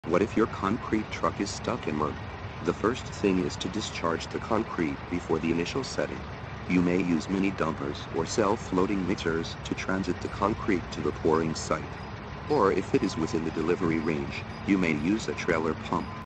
When the Concrete Truck is Stuck in Mud
After surviving for two-weeks wait for your concrete to be delivered, or the driver coming late by making a wrong turn, can you keep calm when you are told the concrete truck is stuck in the mud?

Though frustrated, problems must be solved. The truck is easier to be pulled out when it gets lighter and the concrete should be discharged before its initial setting. A good project manager always has more than one choice.

1. Mini Dumpers.
Mini dumpers are available on many sites. They are convenient to transit concrete to small or narrow corners.

2. Self Loading Mixers.
Self-loading mixers are also compact and flexible. Not only can they mix concrete on site, but also they can be used as small-volume transit trucks.

3. Concrete Pumps.
The pump truck may not perform well on muddy roads. In this case, a trailer pump will do a better job if the pouring site is within its delivery distance.

As a plan B, it is a good idea to reserve a few units of self-loading mixers, for an alternative source of concrete supply. And it can be used for transportation in case of emergency.